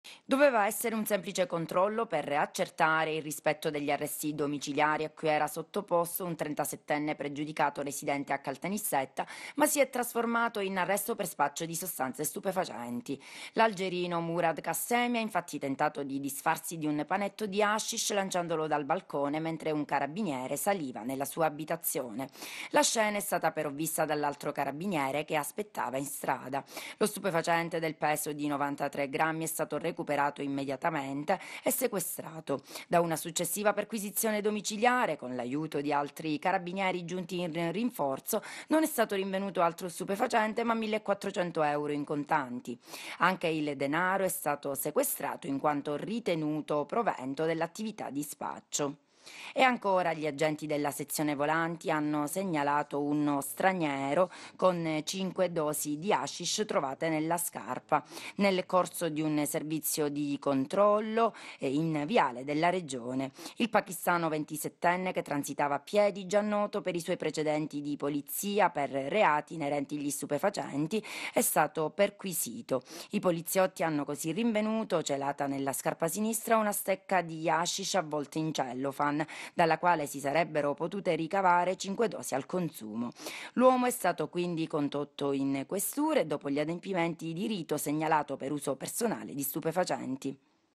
Le segnalazioni dello spaccio
Un arresto per spaccio una segnalazione per hashish